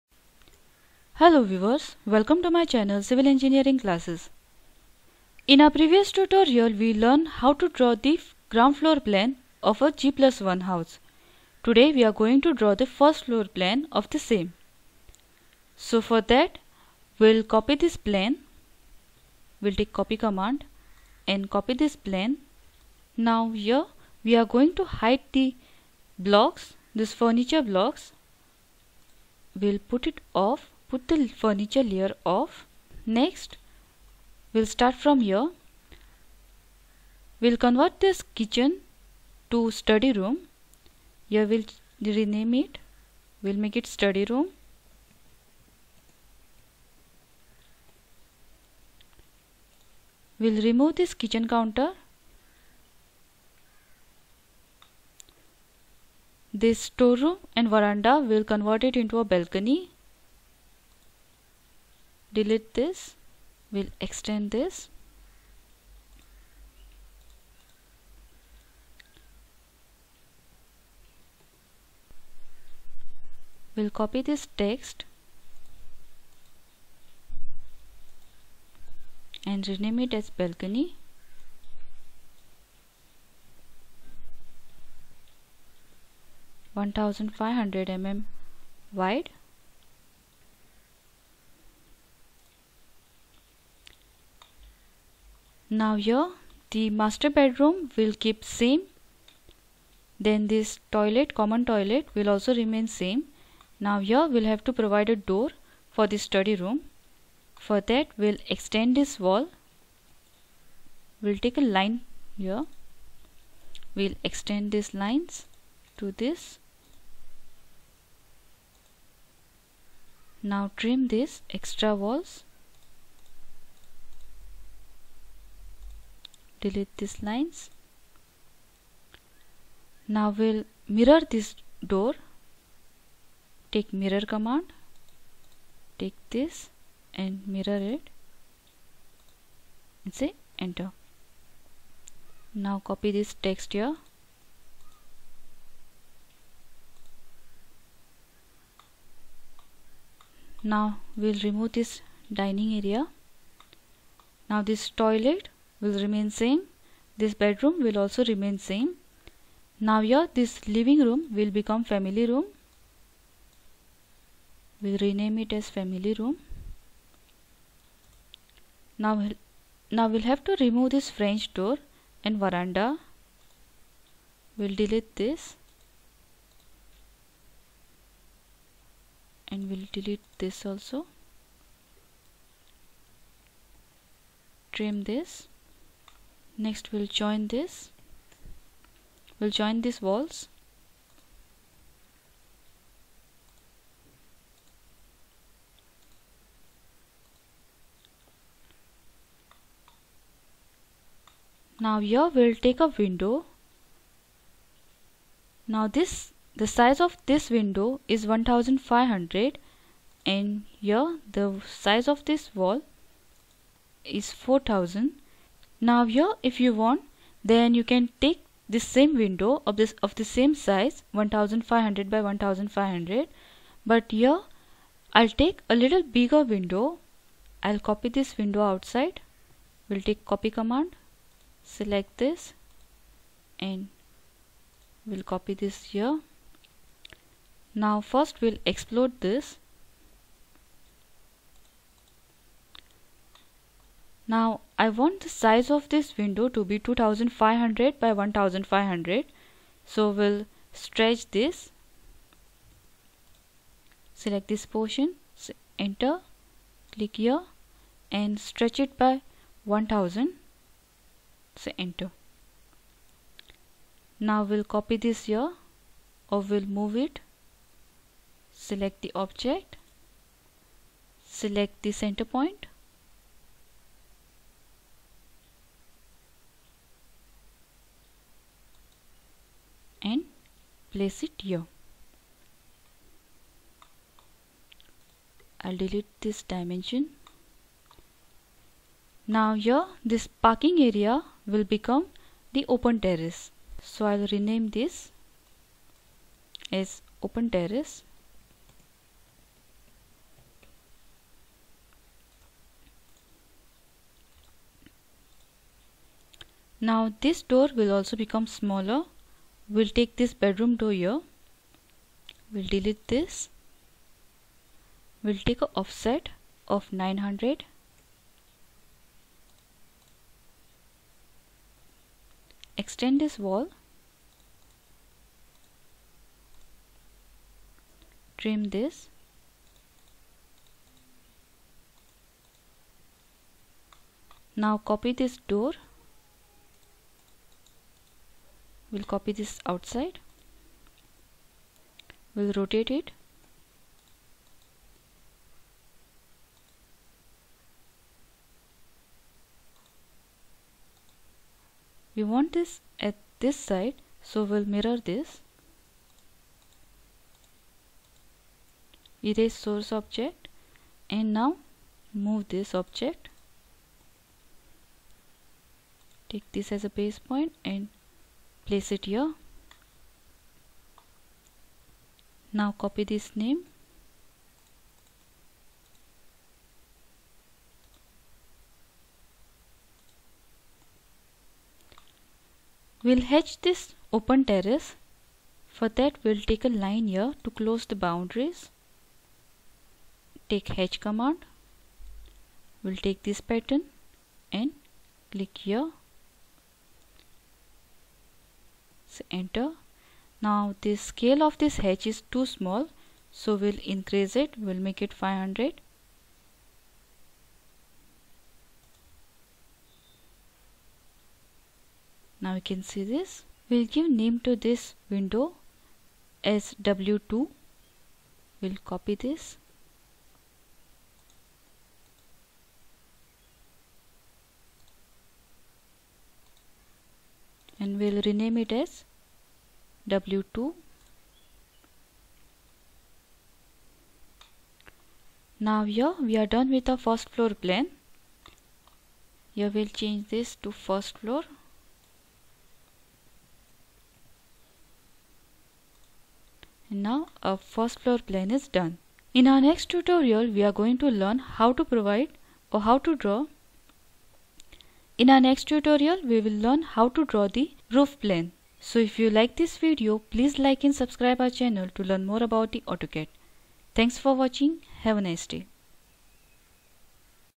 House Plan (Part 3)
In this video u will learn how to draw floor plans and elevations of house plan